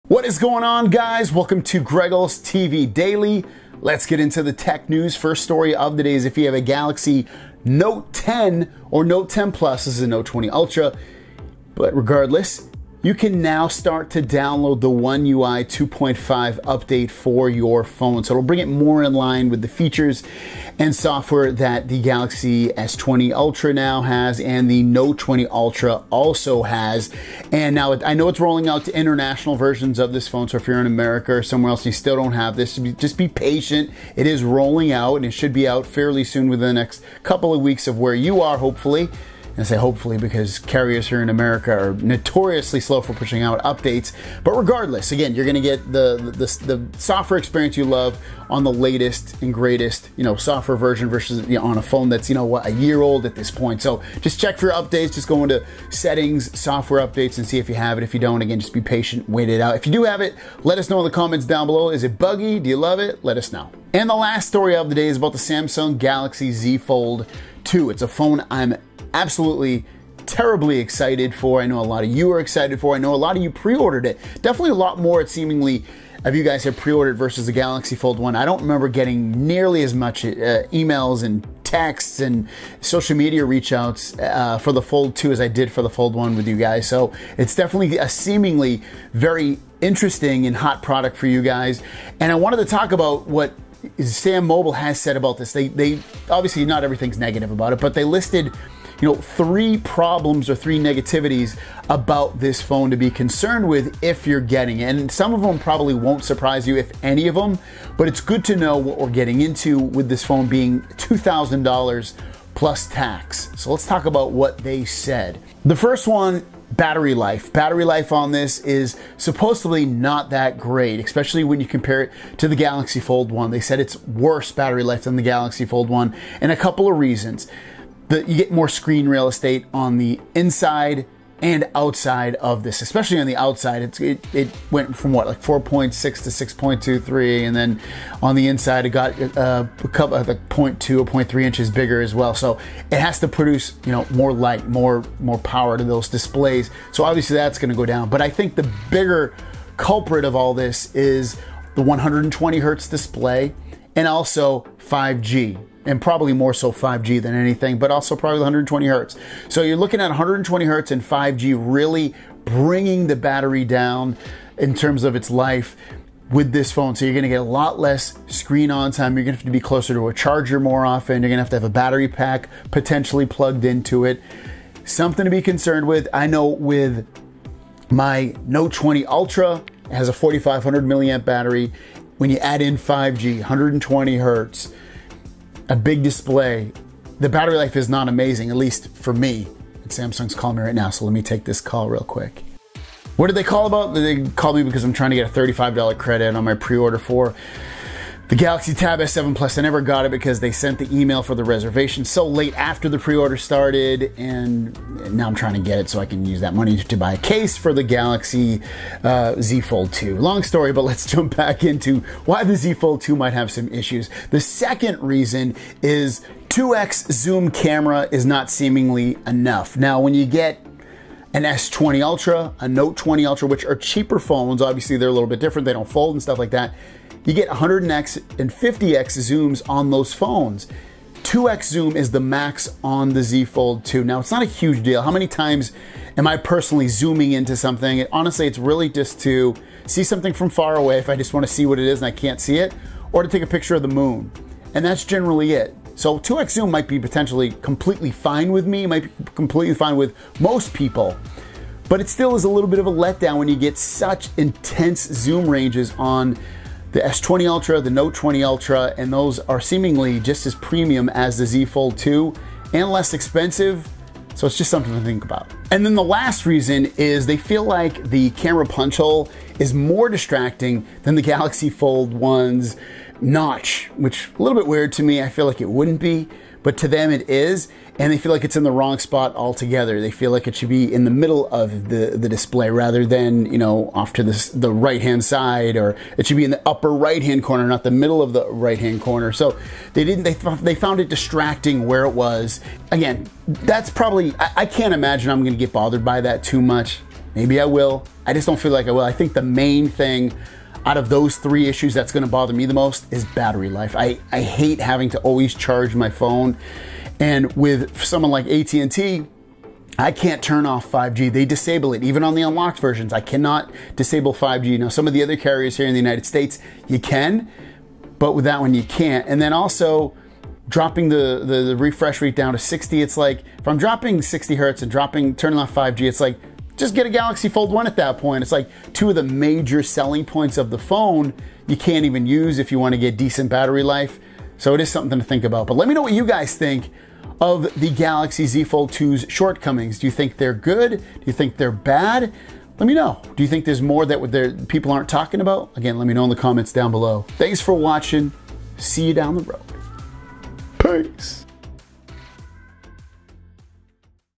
Galaxy Z Fold2 5G: 3 Concerns | Note 10 ONE UI 2.5 Update
Galaxy Z Fold2 5G: 3 Things NOT to Like!? | Note 10 ONE UI 2.5 Update

Mint Mobile  (T-Mobile) $25 12GB Data, Unlimited Talk & Text

Bookmarks
00:00 - Note 10 ONE UI 2.5 Update
01:03 - Galaxy Z Fold2 5G: 3 Things NOT to Like!?
05:36 - Question of the Day

Galaxy Z Fold2 5G: 3 Things NOT to Like!?